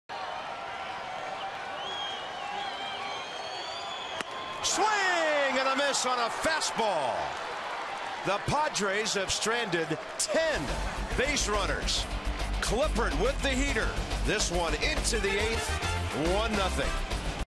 2012/04/26 Clippard escapes a jam
WSH@SD: Clippard escapes bases-loaded jam in seventh